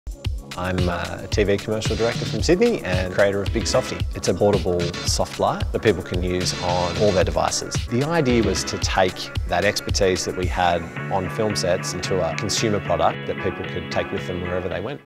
Amazon Prime Day presents BIGSOFTI - Shop Local Campaign 15
Make your content better today.  BIGSOFTI is a high quality, professionally inspired, portable soft light.  Created by Australian TV Commercial director Al Morrow.  Produce great looking photos and video on the go.